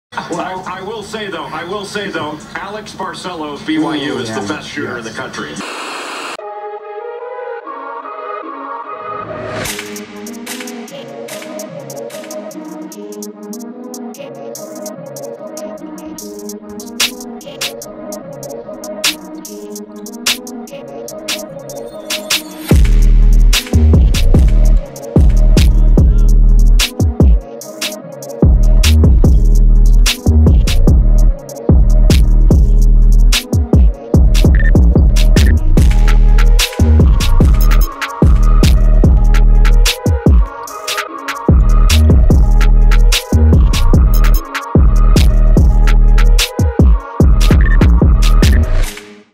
BYU Men's Basketball | Alex Barcello Best Shooter In America
BYU Men's Basketball Guard Alex Barcello is the best shooter in America.